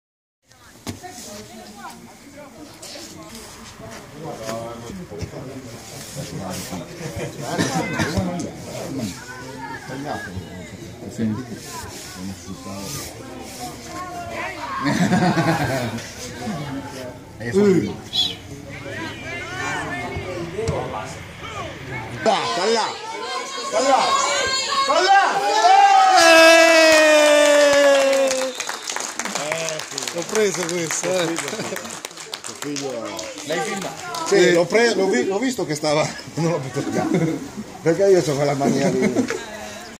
Lombardia - Giovanissimi Provinciali Pavia - Girone A - Giornata 5 - Cassolese vs Virtus Lomellin
I video di Tuttocampo: Lombardia - Giovanissimi Provinciali Pavia - Girone A - Giornata 5 - Cassolese vs Virtus Lomellin